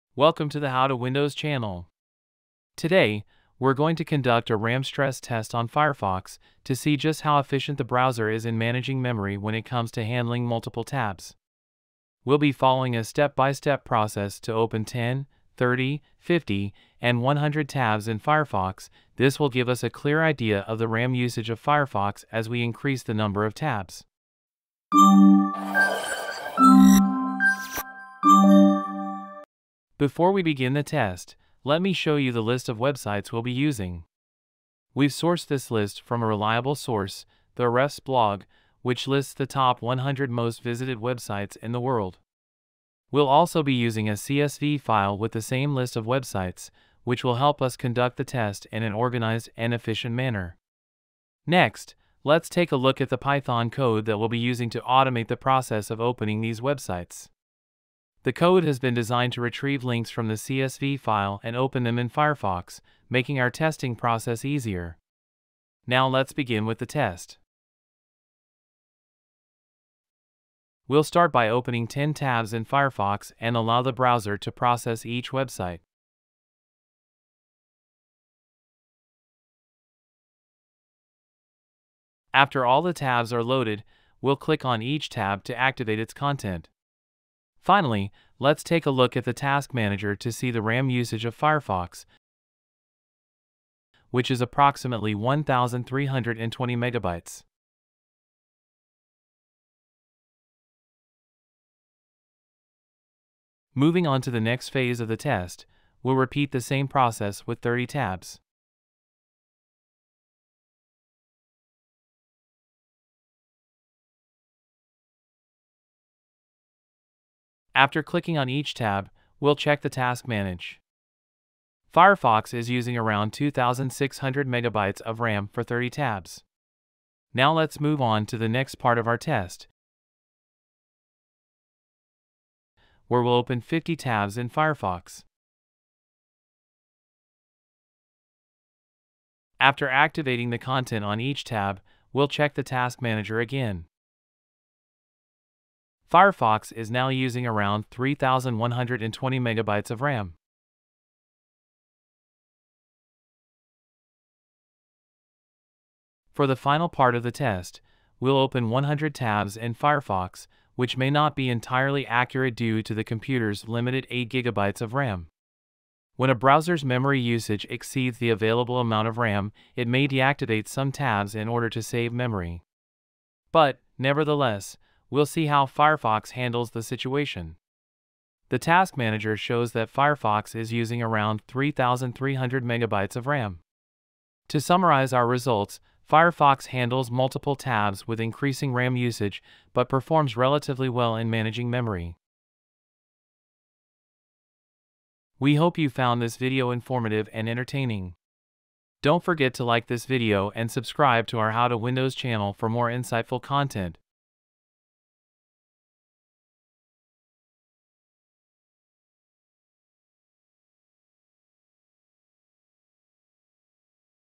100 Tabs vs Mozilla Firefox - Ram Usage Test
In this video, we're putting Mozilla Firefox to the test by opening multiple tabs and seeing how much RAM it uses. We'll start with 10 tabs and work our way up to 100, to give you a clear idea of how efficient Firefox is at managing memory.

Throughout the video, we'll be using the Task Manager to monitor the RAM usage of Firefox, so you can see how much memory is being used at each stage of the test.

To make the process easy and efficient, we've created a Python code to automate opening the websites.

By the end of the video, you'll have a clear idea of how Firefox handles multiple tabs and manages memory.